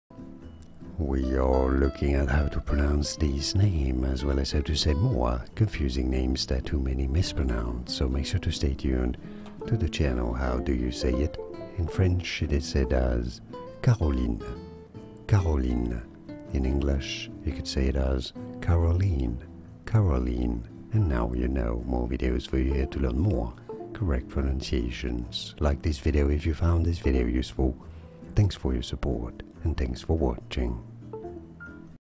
How to Pronounce Caroline (French)
This video shows you How to Pronounce Caroline (French), pronunciation guide.
00:00 - Pronunciation Intro
00:08 - How to Pronounce Caroline (French)
00:20 - More Difficult Pronunciations